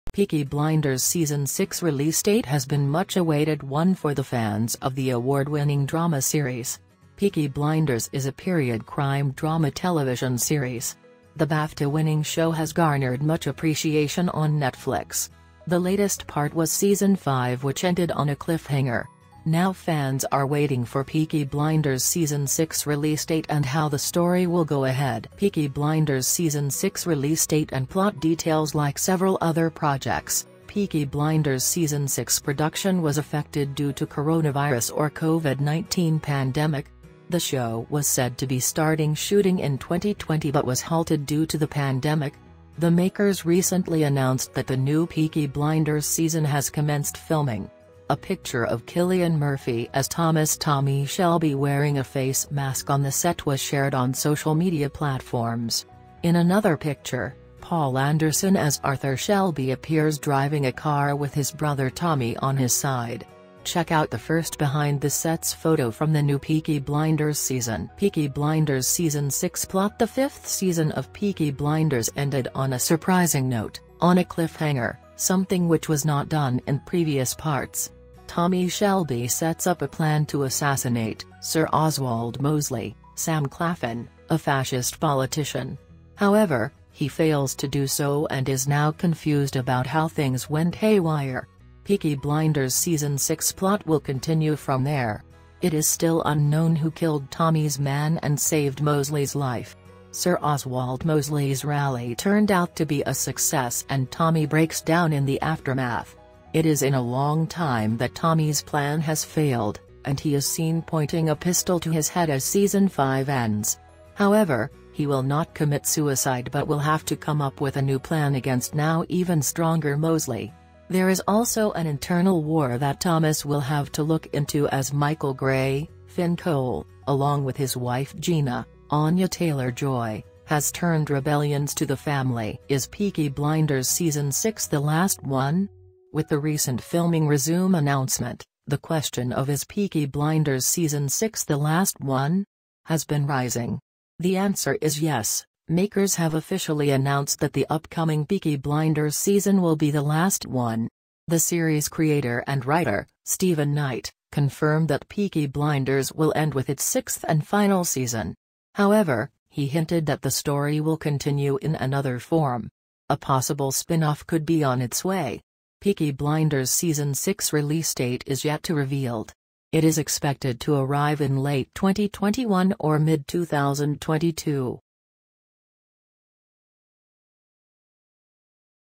'Peaky Blinders' Season 6 Release Date And What To Expect From It
in this video i have talked about Peaky Blinders Season 6: Release Date Expected also a new SHOCKING Announcement Revealed by the officials

PeakyBlinders season 6 trailer i not available until the date of this video

TAGS -